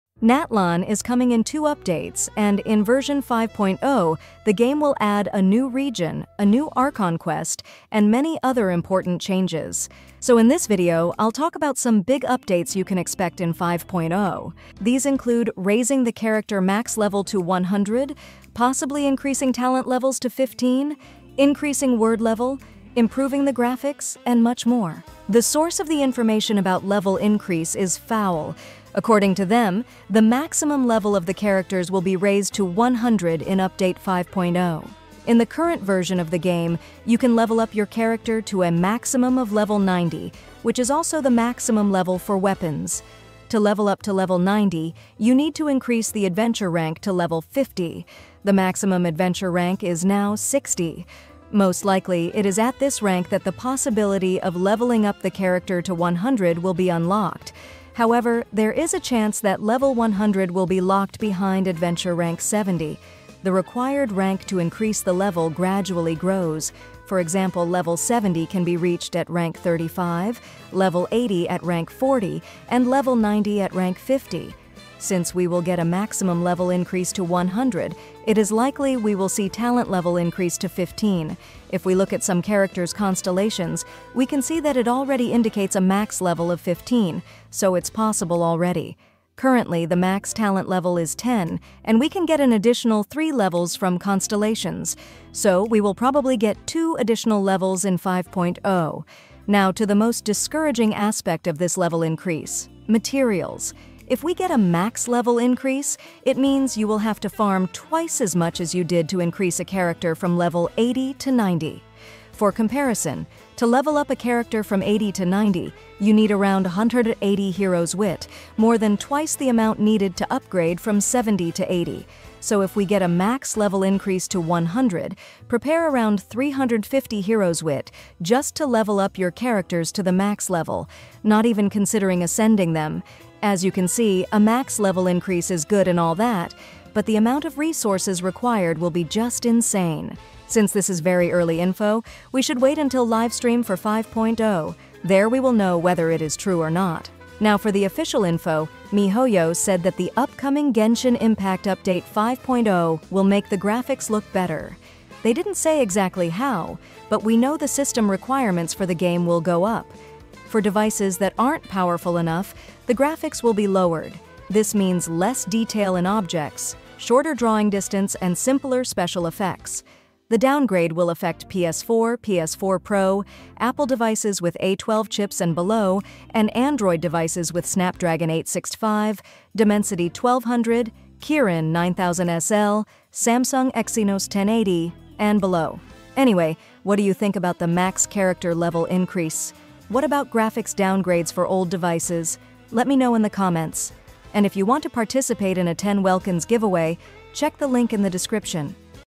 CHARACTERS LEVEL INCREASE TO 100, TALENTS LEVEL TO 15 AND BETTER GRAPHICS IN NATLAN - Genshin Impact
Natlan is coming in two updates, and in version 5.0, the game will add a new region, a new Archon quest, and many other important changes.

So In this video, I'll talk about all the big updates you can expect in 5.0. These include raising the character max level to 100, possibly increasing talent levels to 15, increasing word level, improving the graphics, and much more.

The source of the information about level increase is foul. According to them, the maximum level of the characters will be raised to 100 in  update 5.0.

In the current version of the game, you can level up your character to a maximum of level 90, which is also the maximum level for weapons.

To level up to level 90, you need to increase the Adventure Rank to level 50. The maximum Adventure Rank is now 60. Most likely, it is at this rank that the possibility of leveling up the character to 100 will be unlocked.

However, there is a chance that level 100 will be locked behind Adventure Rank 70. The required rank to increase the level gradually grows: for example, level 70 can be reached at rank 35, level 80 at rank 40, and level 90 at rank 50.

Since we will get a maximum level increase to 100, it is likely we will see talent level increase to 15. If we look at some characters' constellations, we can see that it already indicates a max level of 15, so it's possible already in the game. Currently, the max talent level is 10, and we can get an additional 3 levels from constellations. So we will probably get 2 additional levels in 5.0.

Now to the most discouraging aspect of this level increase - materials.

If we get a max level increase, it means you will have to farm twice as much as you did to increase a character from level 80 to 90. For comparison, to level up a character from 80 to 90, you need around 180 Hero's Wit, more than twice the amount needed to upgrade from 70 to 80. So if we get a max level increase to 100, prepare around 350 Hero's Wit just to level up your characters to the max level, not even considering ascending them.

As you can see, a max level increase is good and all that, but the amount of resources required will be just insane.

Since this is very early info, we should wait until livesream for 5.0, there we will know wether it is true or not.

By the way, Update 5.0 itself will be released on August 28, 2024.

Now for the official info: miHoYo said that the upcoming Genshin Impact update 5.0 will make the graphics look better. They didn’t say exactly how, but we know the system requirements for the game will go up.

For devices that aren't powerful enough, the graphics will be lowered. This means less detail in objects, shorter drawing distance, and simpler special effects.

The downgrade will affect PS4, PS4 Pro, Apple devices with A12 chips and below, and Android devices with Snapdragon 865, Dimensity 1200, Kirin 9000SL, Samsung Exynos 1080, and below.